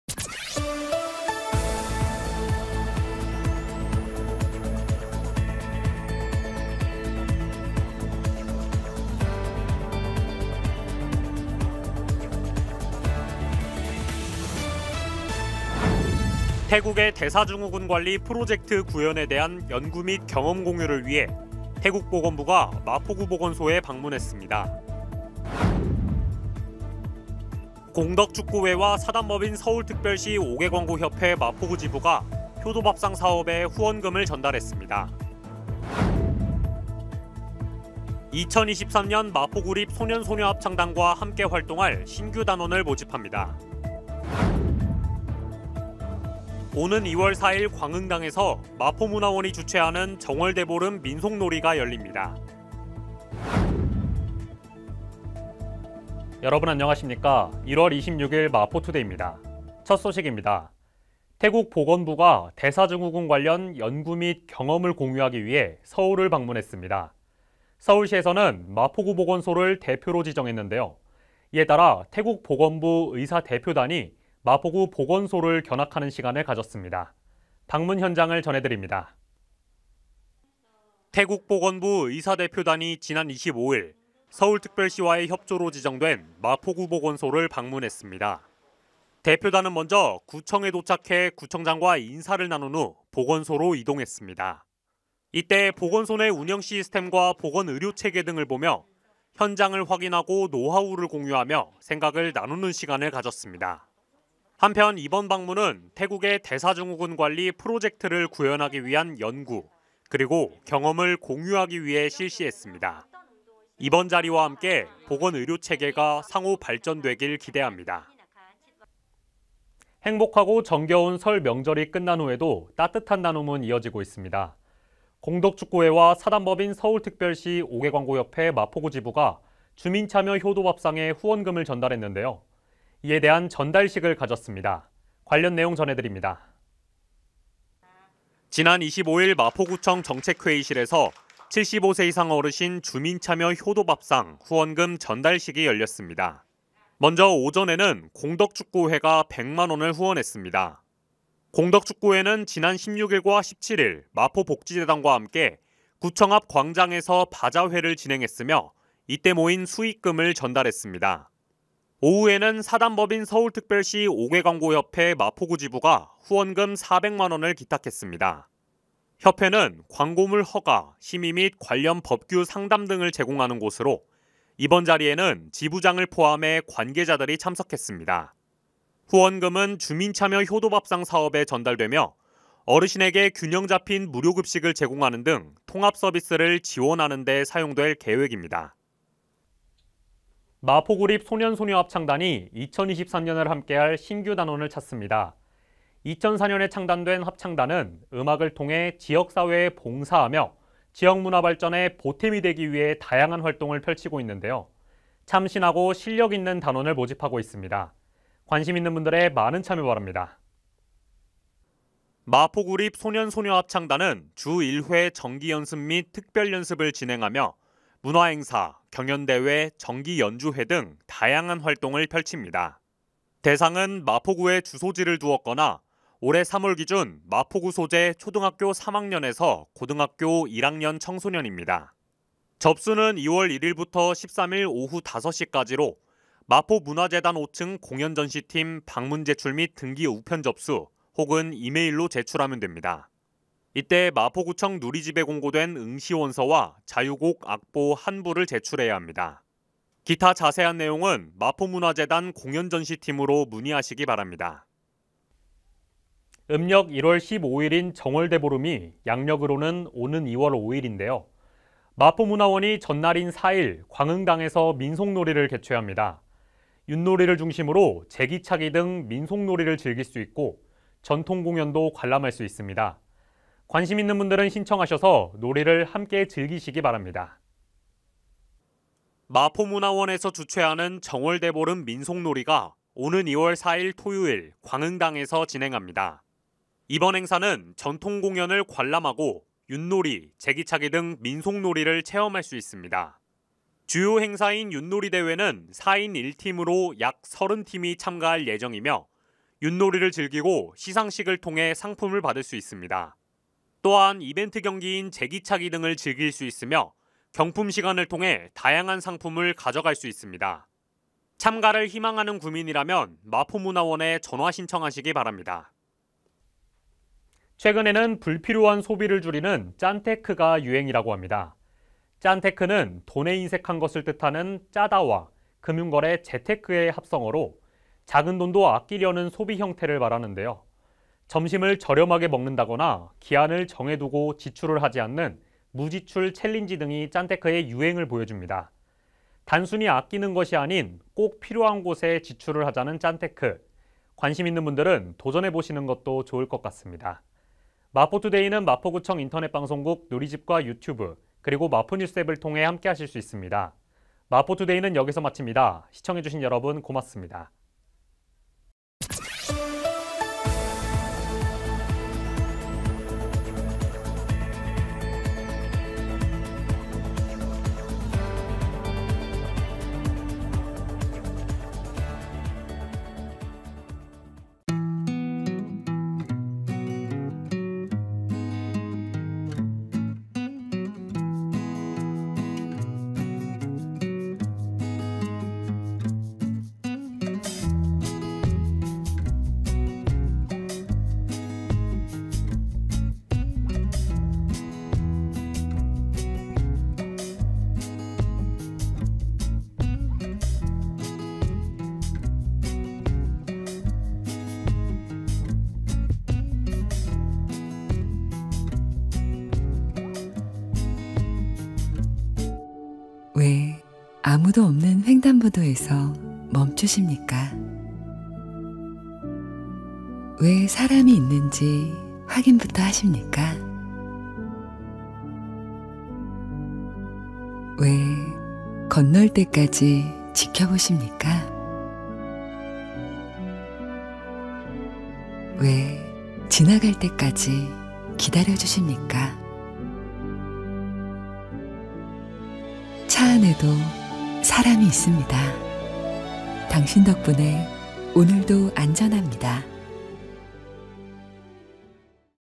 태국 보건부, 연구 및 경험 공유 위한 보건소 방문 外 / 마포투데이 (23. 1. 26)
00:00​​​​​​ ~ 00:52 헤드라인
00:53 ~ 01:58 태국 보건부, 연구 및 경험 공유 위한 보건소 방문
01:59 ~ 03:09 계속되는 후원 릴레이! 효도밥상 성금 기탁
03:10 ~ 04:15 합창의 매력에 빠져보자! 마포구립소년소녀합창단 모집
04:16 ~ 05:16 신나는 윷놀이 한 판! 정월대보름 민속놀이 모이세요
05:17 ~ 06:14 클로징 멘트
06:15 ~ 07:32 ​마포구 안내